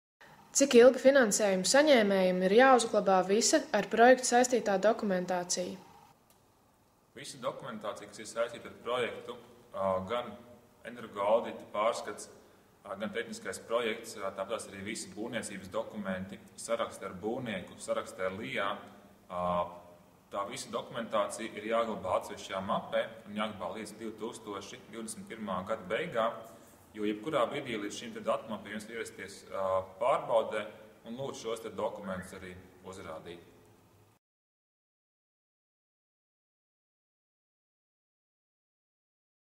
Cik ilgi finansējuma saņēmējam ir jāuzglabā visa ar projektu saistītā dokumentācija?
Latvijas Investīciju un attīstības aģentūra pieņem pieteikumus ERAF aktivitātē "Daudzdzīvokļu māju siltumnoturības uzlabošanas pasākumi". Šīs aktivitātes ietvaros daudzdzīvokļu māju dzīvokļu īpašnieki var saņemt 50-60% līdzfinansējumu ēkas renovācijai. Papildus informācija